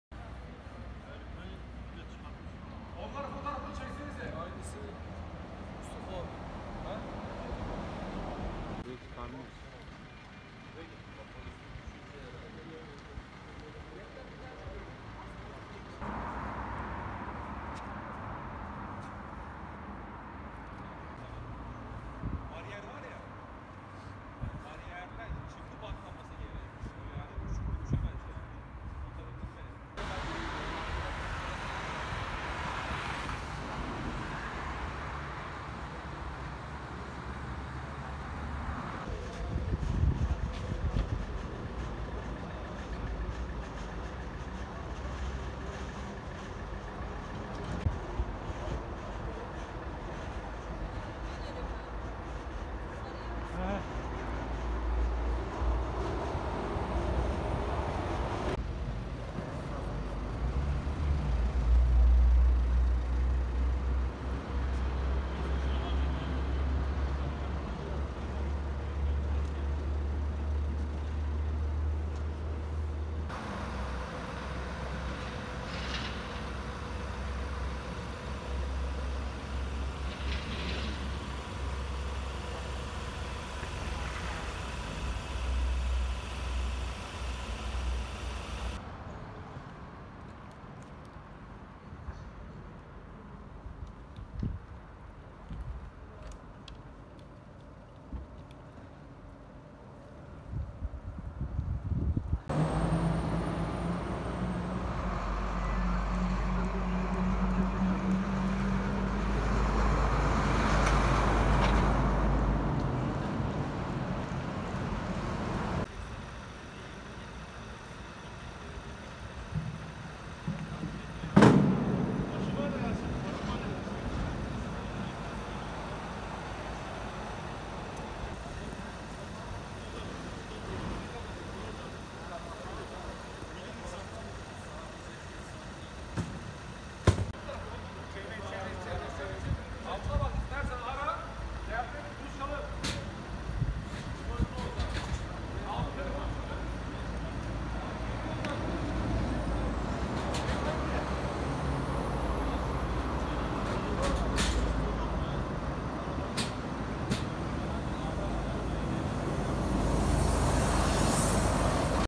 Köprüden tünele düşen yaşlı adam hayatını kaybetti
Derince ilçesinde üzerinden kimlik çıkmayan yaşlı adam, köprüden tünele düşerek hayatını kaybetti.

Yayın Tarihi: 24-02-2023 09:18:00